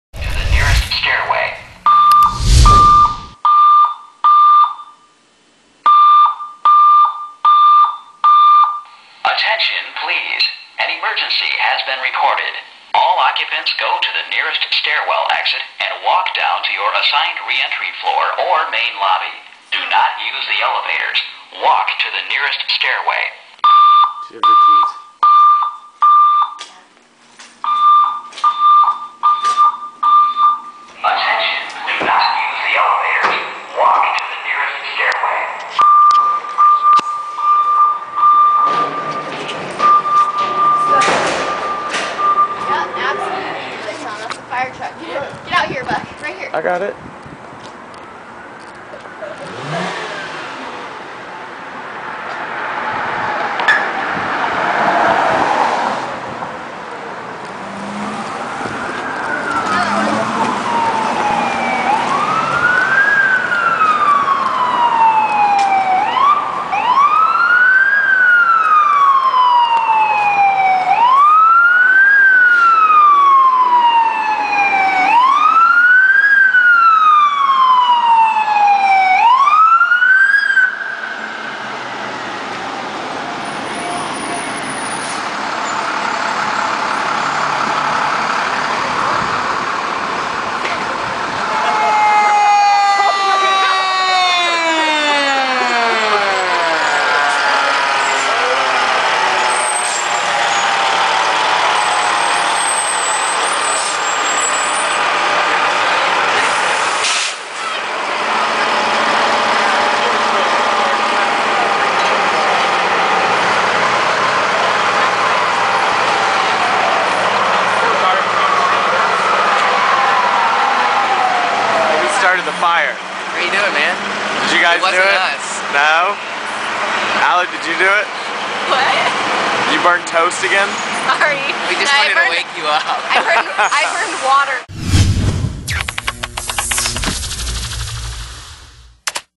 FIRE!
One Bad Cook made a huge ordeal for BuckHeads entire Apt!